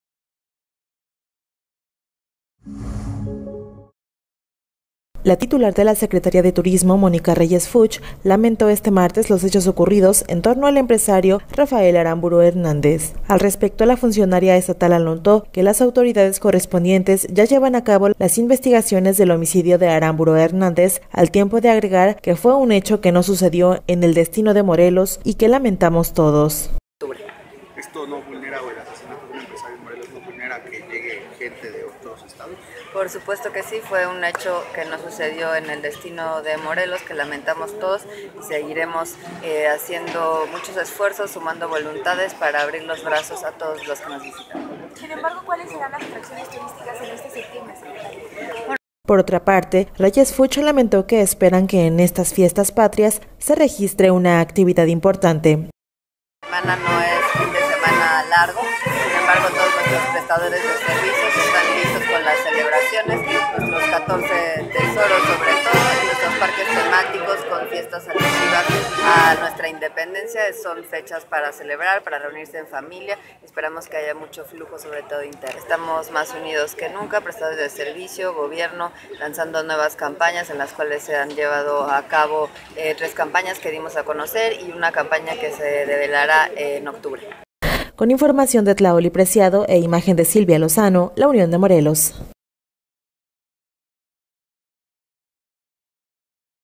Lamenta secretaria de Turismo hechos ocurridos en torno a empresario asesinado
La titular de la Secretaría de Turismo, Mónica Reyes Fuchs, lamentó este martes los hechos ocurridos en torno al empresario Rafael Arámburu Hernández.